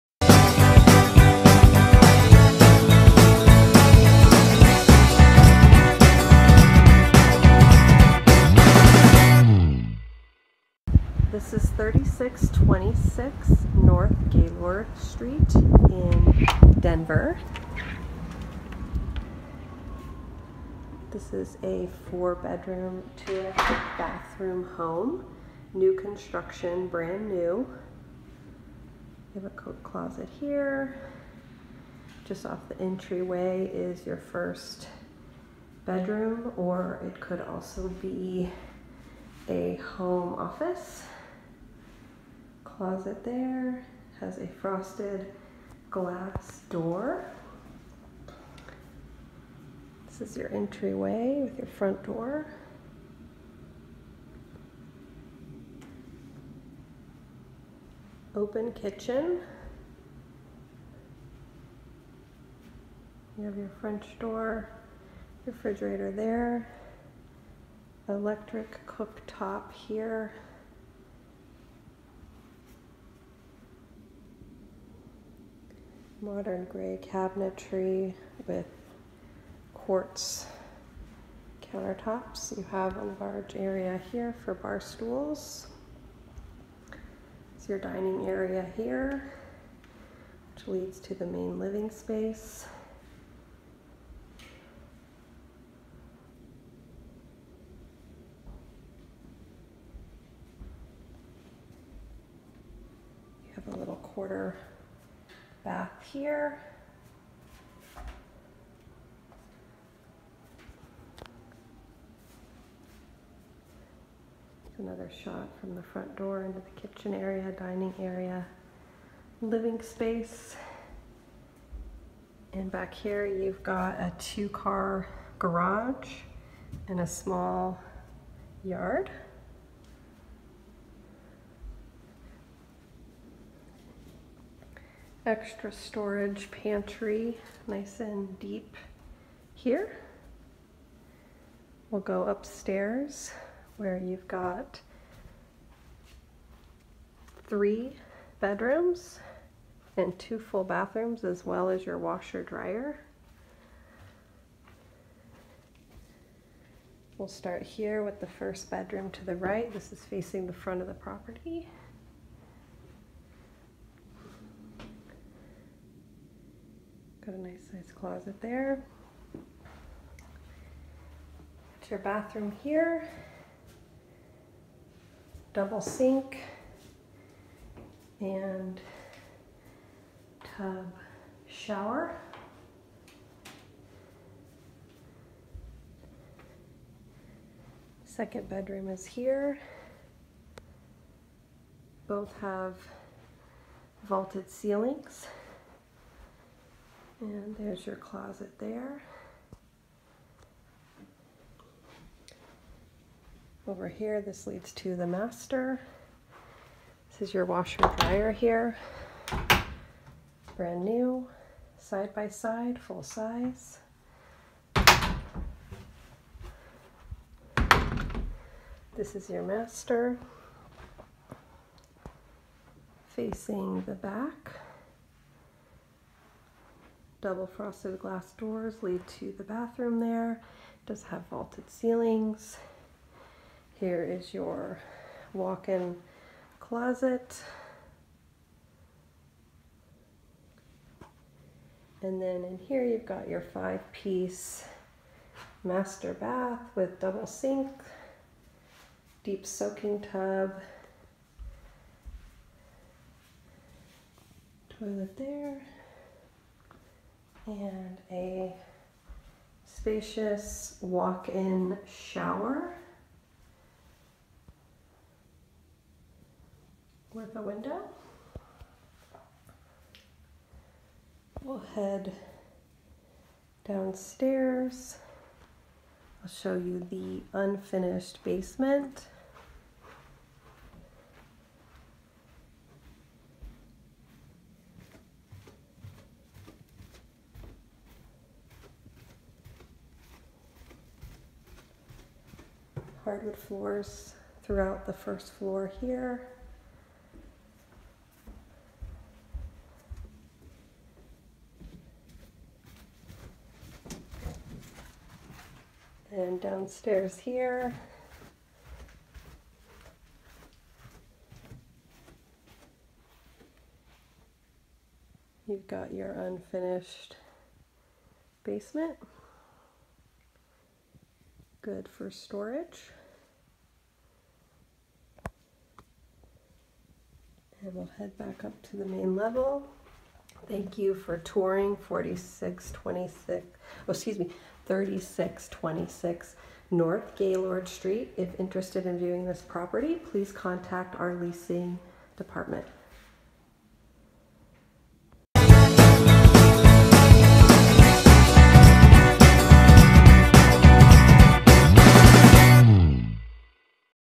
Denver Homes for Rent 4BR/2.5BA - 3626 N Gaylord Street by Grace Property Management & Real Estate
This lovely property is perfect for you! It features 4 bedrooms, 2.5 bathrooms, and a garage. The living/dining area has windows that let in tons of natural light. The open kitchen has Quartz countertops, storage cabinets, and is equipped with major appliances. All of the bedrooms are ample in size and have built-in closets. The first bedrooms can be used as an office space. The other 3 bedrooms are located upstairs. The master suite has a walk-in closet and a private bathroom. The private bathroom boasts a soak-in tub, a shower, and dual vanities. Other features of this stunning property include a laundry area with a washer and dryer and a yard for your outdoor activities. Hurry! This gem is a must-see.

Looking for a property manager in Denver? We do property management!

2200 E 104th Ave UNIT 105, Thornton, CO 80233